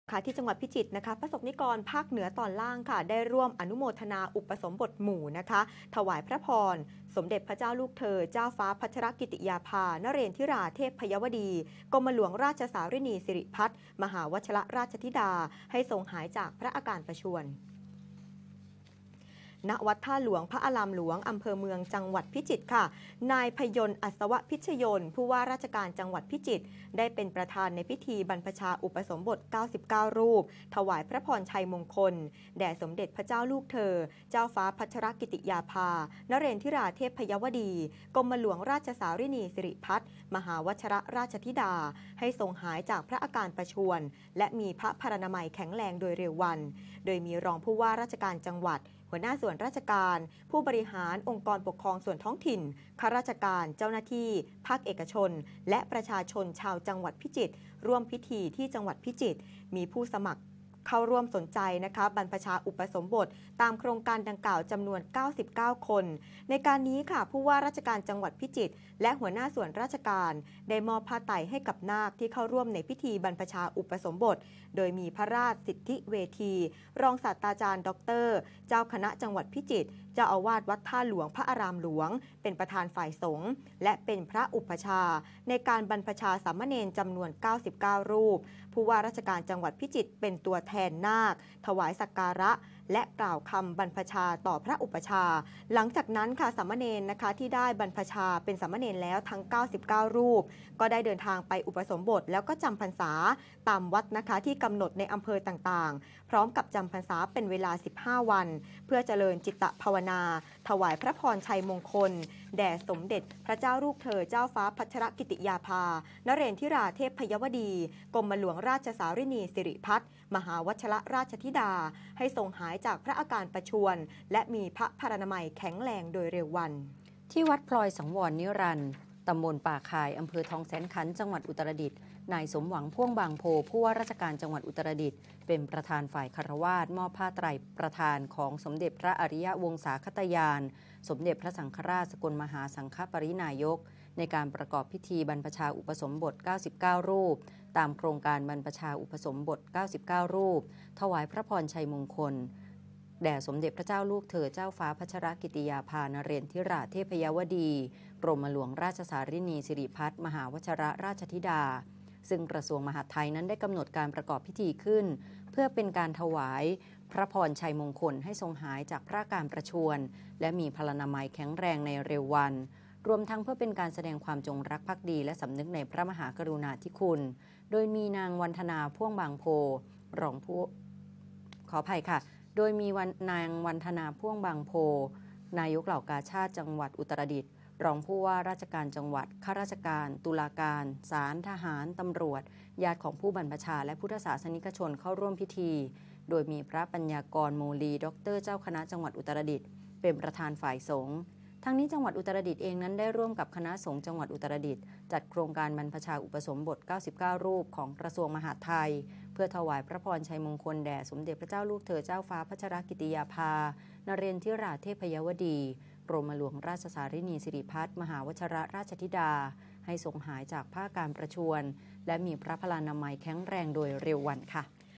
พสกนิกรภาคเหนือตอนล่าง ร่วมอนุโมทนาอุปสมบทหมู่ถวายพระพรสมเด็จเจ้าฟ้าพัชรกิติยาภาฯ
พสกนิกรภาคเหนือตอนล่าง  ร่วมอนุโมทนาอุปสมบทหมู่ ถวายพระพร สมเด็จพระเจ้าลูกเธอ เจ้าฟ้าพัชรกิติยาภา นเรนทิราเทพยวดี กรมหลวงราชสาริณีสิริพัชร มหาวัชรราชธิดา  ให้ทรงหายจากพระอาการประชวร